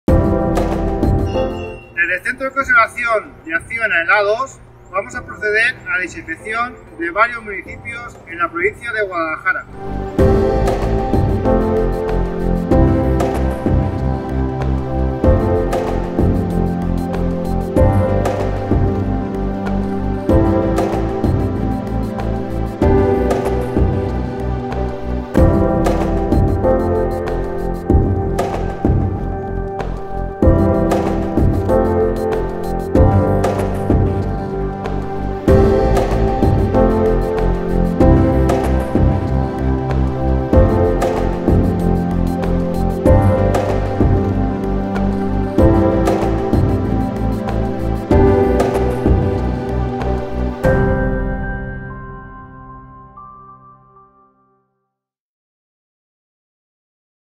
ACCIONA desinfecta las vías públicas de Guadalajara
ACCIONA mantiene fuertes vínculos con el entorno donde desarrolla su actividad. Adecua su maquinaria para poder realizar labores de desinfección en la situación de emergencia sanitaria actual, ayudando a contener la propagación de la COVID-19.